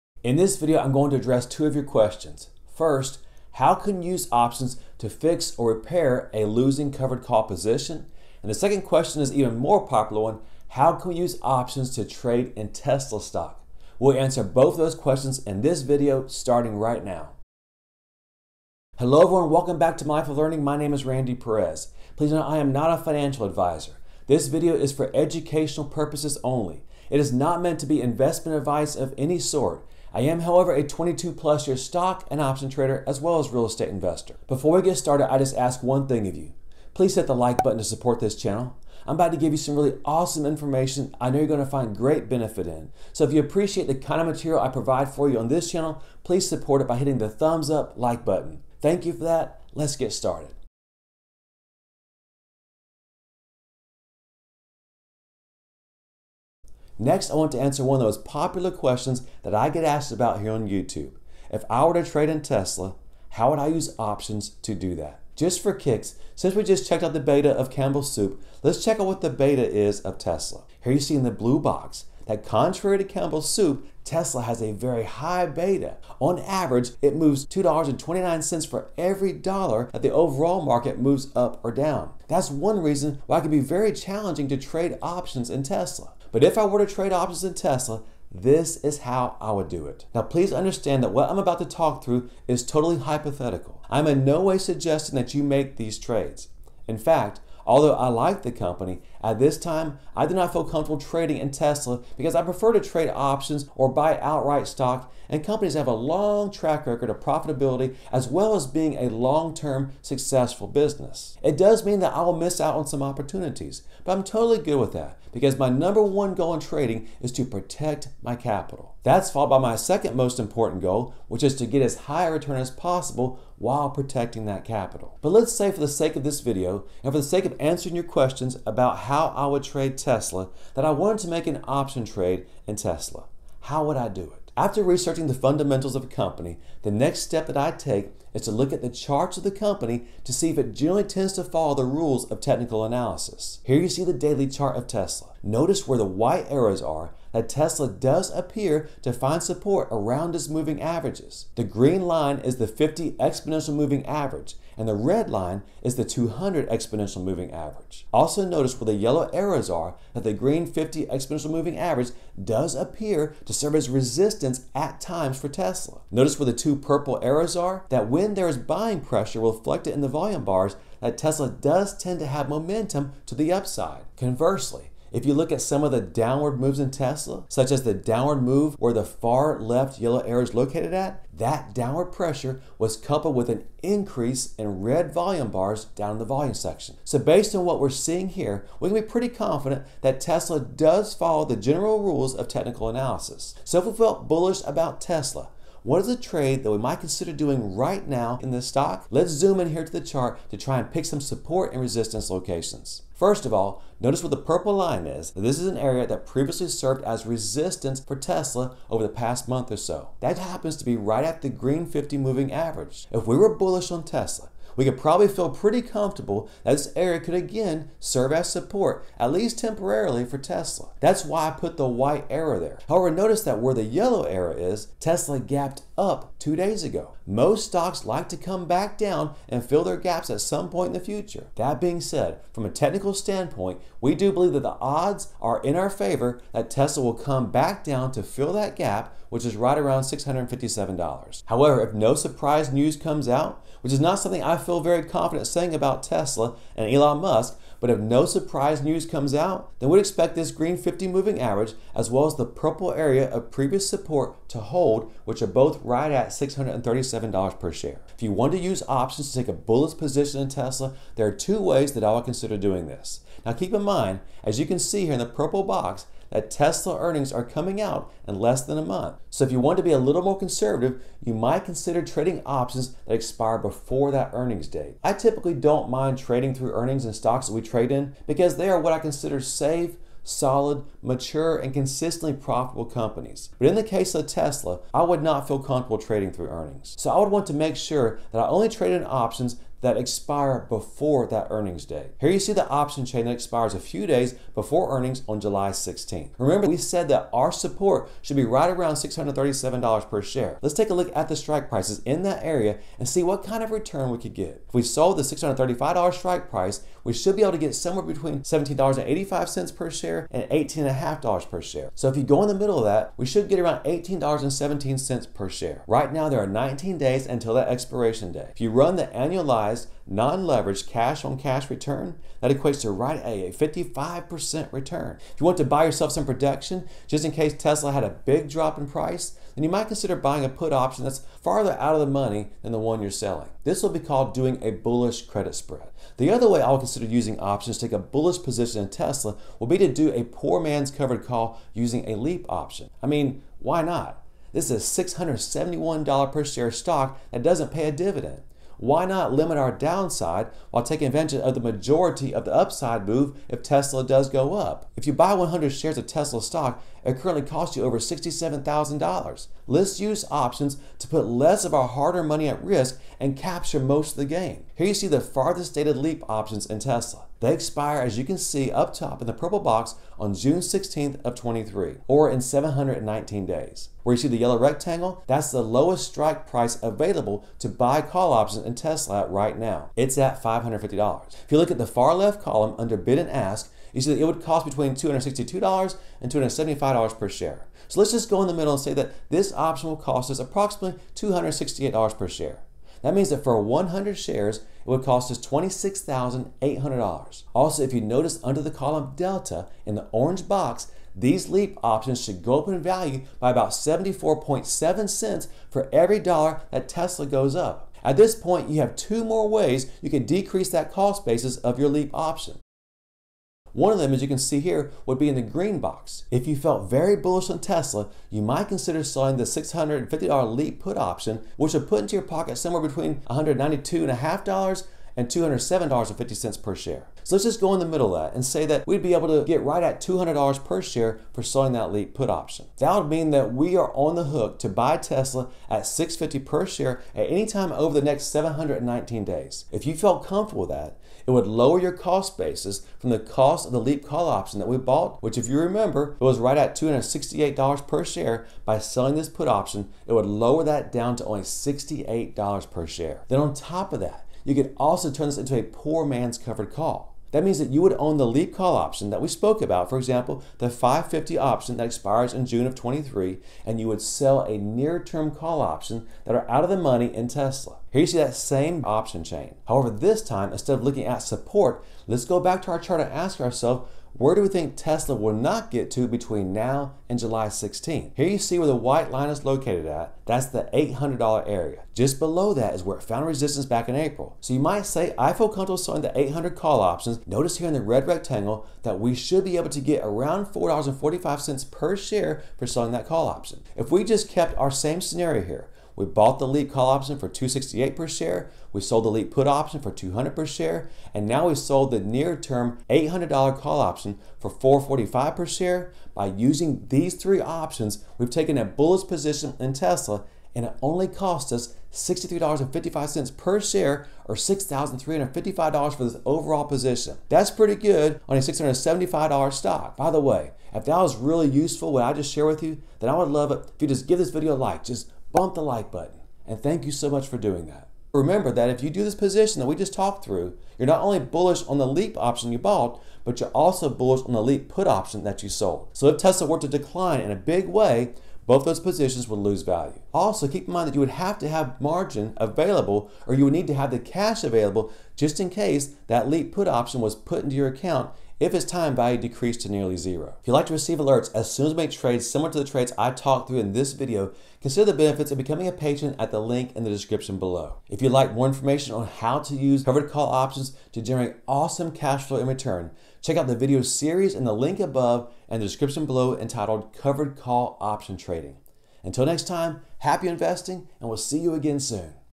How to Sell Options in TSLA
💰 BLACK FRIDAY SALE until midnight on Tuesday, 12/3/2024 eastern!

⏰Chapters ⏰
0:00 Into
2:05 Is TSLA a good stock to trade using technical analysis
4:18 Selling put options in TSLA (possible option trade #1)
5:36 Buying LEAP call options TSLA (possible option trade #2)
6:48 Selling LEAP put options in TSLA (possible option trade #3)
7:33 Poor man's covered call in TSLA (possible option trade #4)
8:13 Results of combining 3 of these option trading strategies

🔴 PART 1 of this video can be found here - How to Fix a Covered Call Option

In this video, I’m going to answer one of the most popular questions that I get asked: How can we use options to trade in Tesla stock?

Here you see the daily chart of Tesla. Based on what we’re seeing here, we can be pretty confident that Tesla tends to follow the general rules of technical analysis. If we felt bullish on Tesla, what is a trade we might consider doing tomorrow, Monday, June 28th? Let’s zoom in here to the chart to pick some support & resistance.

Notice this area that previously served as resistance for Tesla over the past month or so. That just so happens to be right at the green 50 moving average. If we were bullish on Tesla, we could feel pretty comfortable that this area could again serve as support for Tesla.

If we sold the $635 strike price put option, we should be able to get around $18.17 per share. If you run the annualized, non-leveraged cash on cash return, that equates to right at a 55% return. If you wanted to buy yourself some protection, just in case Tesla had a big drop in price, you might consider buying a put option that is farther out of money than the one you’re selling. This would be called doing a bullish credit spread.

Another way I would consider using options to take a bullish position in Tesla is to buy a LEAP call option. Once you’ve done that, there are two more ways to decrease the cost basis of our LEAP call option.

One is you might consider selling the $650 LEAP put option. That would put right at $200 per share into your pocket. On top of that, you could also turn this into a PMCC by selling the June 16th $800 call option. We should be able to get around $4.45 per share for that option.

Notice our out of pocket cost if we traded the above option scenario: bought the LEAP call option for $268 per share, sold the LEAP put option for $200 per share & sold the near term $800 call option for $4.45 per share. With these 3 options, we've taken a bullish position in Tesla that will only cost us $63.55 per share cash. That’s great on a $675 stock!

Happy investing!
Randy Perez